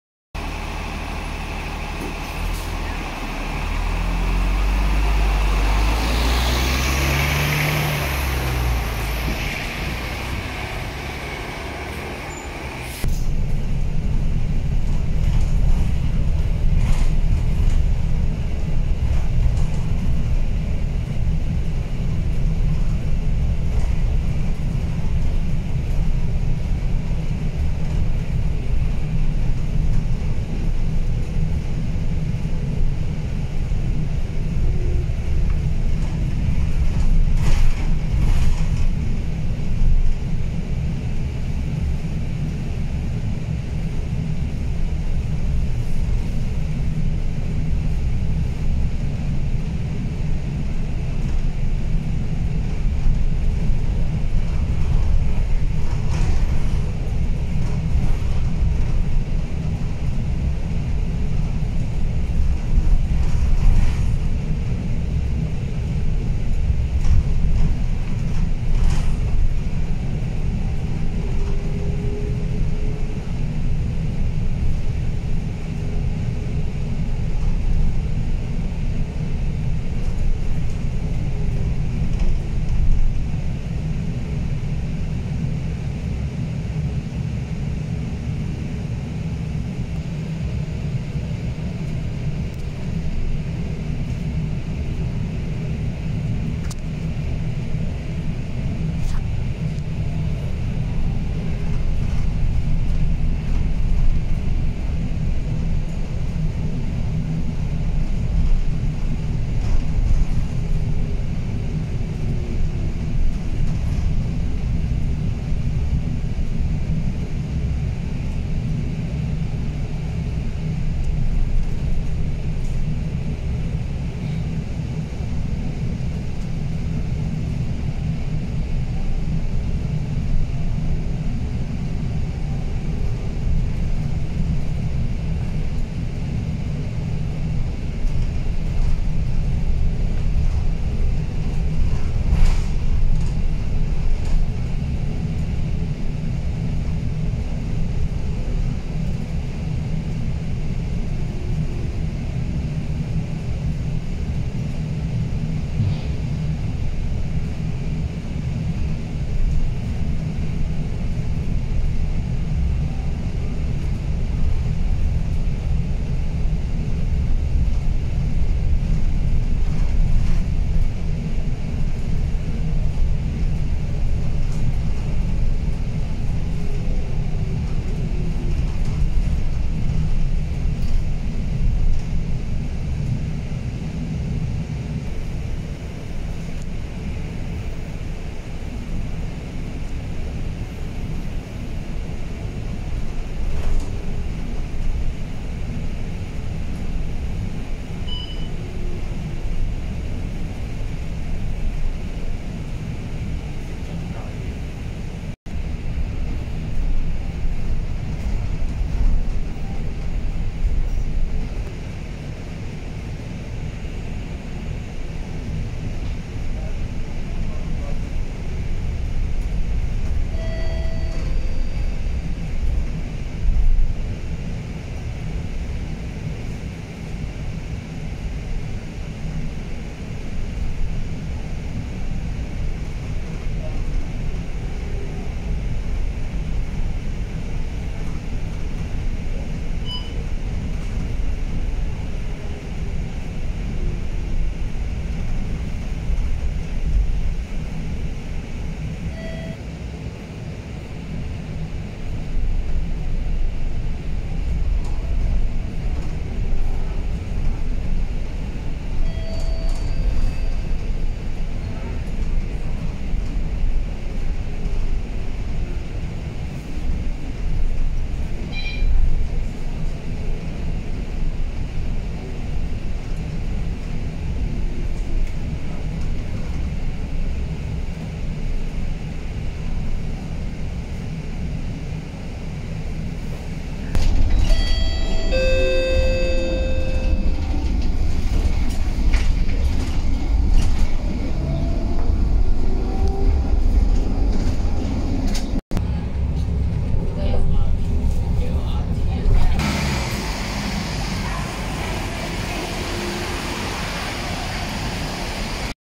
(SMRT) Electric- SG7002L on Service 983 - Yutong ZK6125BEVGS E12DD
SRTW films SG7002L, a fully electric Yutong ZK6125BEVGS E12DD, on Service 983.

Permanent Deployment Status: 983
Depot: Kranji Depot